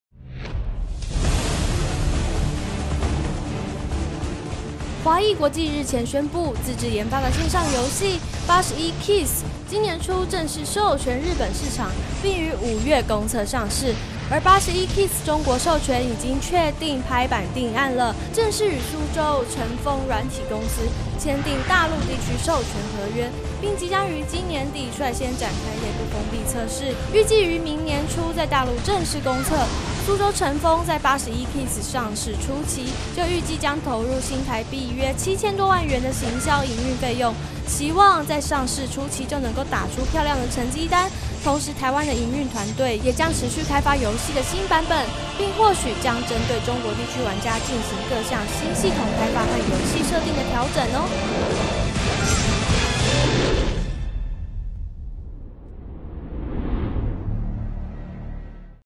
電玩宅速配100914_《81 Keys》大陸授權底定 預計明年上市
華義國際日前宣佈，自製研發的線上遊戲《81 Keys》，今年初正式授權日本市場，並於五月公測上市，而《81 Keys》中國授權已確定拍板定案，正式與蘇州晟豐軟體公司，簽訂大陸地區授權合約，並即將於今年底率先展開內部封閉測試，預計於明年初在大陸正式公測。，蘇州晟豐在《81 Keys》上市初期，就預計將投入約新台幣7000多萬元的行銷營運費用，期望在上市初期就能打出漂亮成績單。同時台灣的營運研發團隊，也將持續開發遊戲新版本，並或許將針對中國地區玩家，進行各項新系統開發和遊戲設定調整。